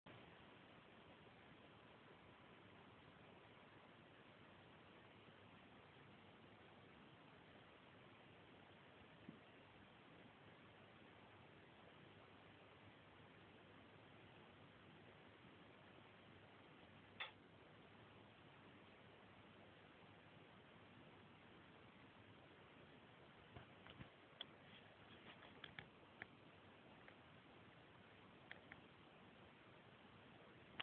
poruszająca się lampa
czy ta lampa rzeczywiście samoistnie się porusza?